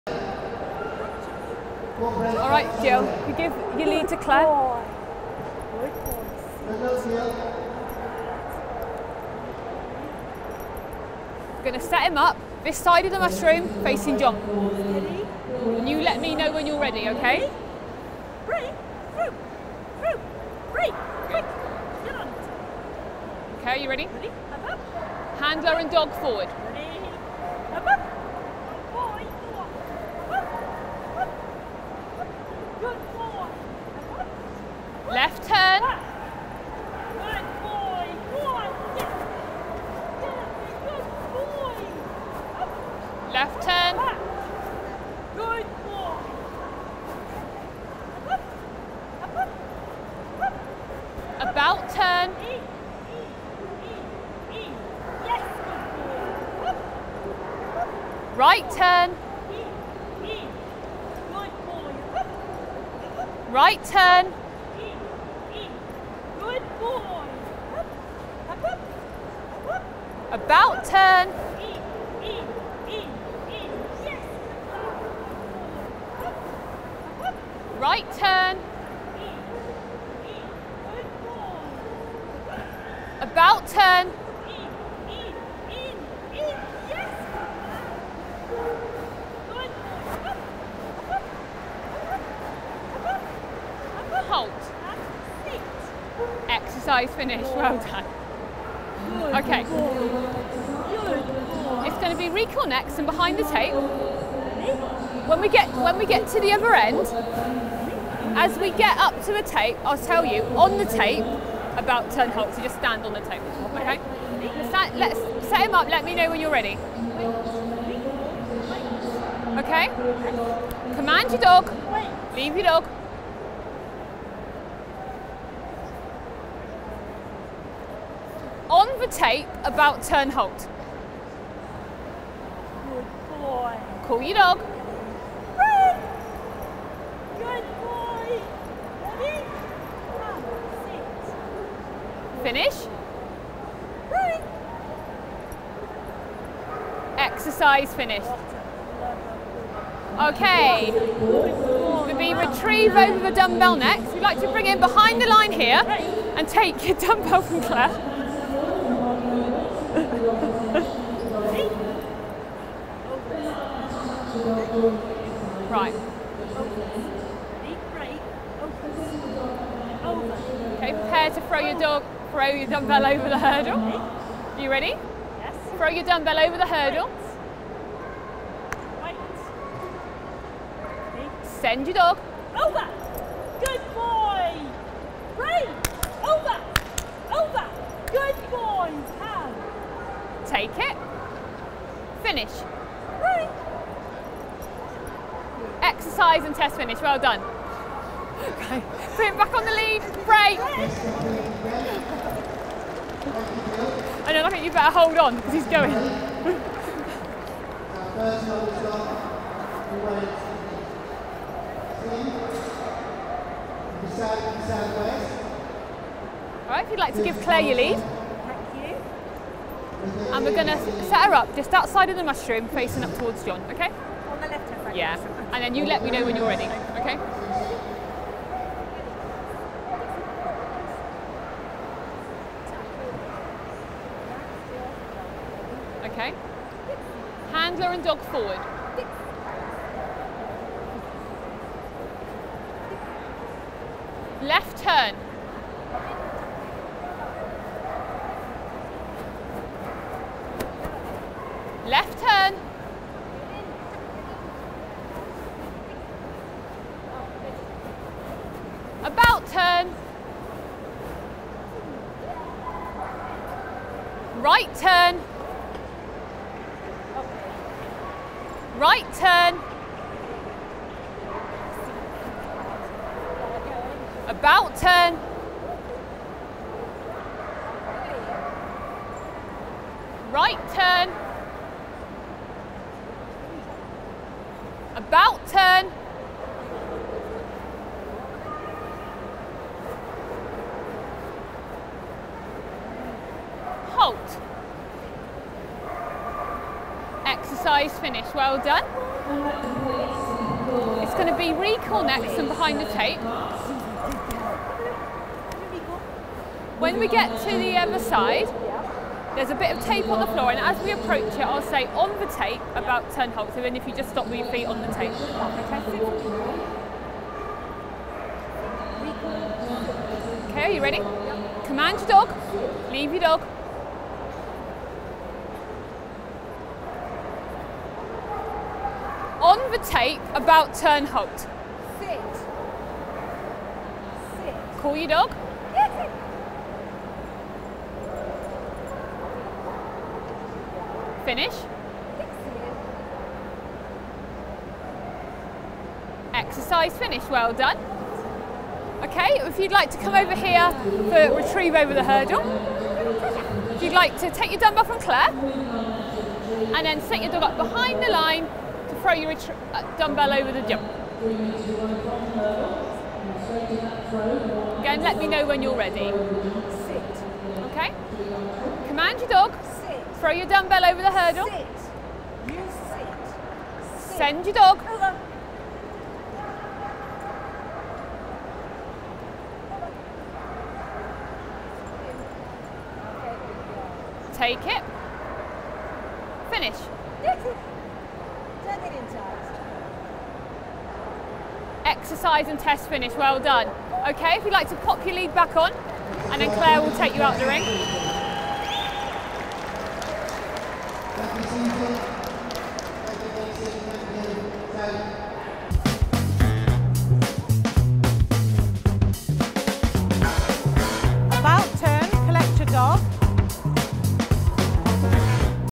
Inter-Regional Obedience - Novice Class - Part 1 | Crufts 2014
Part 1/4 from the Novice Class group at the Crufts 2014 Inter-Regional Obedience Competition.

Due to a camera technical issue, regrettably not all competitors appear in this competition.

Competitors missing are the 1st five Novice competitors in the Inter-Regional:
• Gayle Evans
• Teresa Mills
• Paul Tennant
• Lynn Griffin
• Gill Walker